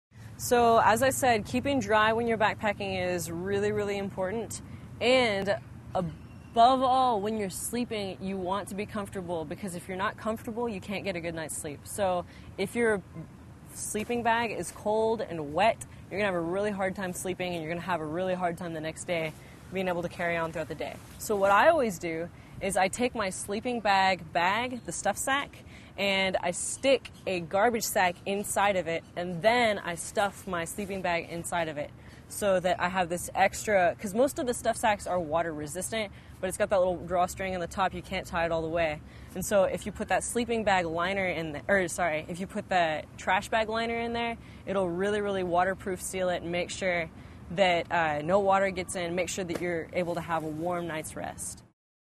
Backpacking Rain Gear : Backpacking Rain Gear: Keeping Sleeping Bag Dry
If a sleeping bad is uncomfortable, it's hard to get any sleep. Learn how to keep sleeping bags dry and what rain gear to bring on backpacking trips in this free camping video.

Expert: Megan Rouch
Bio: Megan Rouch enjoys off-road unicycling. She has been riding unicycles for eight years. Miss Rouch has instructed unicycling and juggling at the Kent Cummins Magic Camp for the past seven years.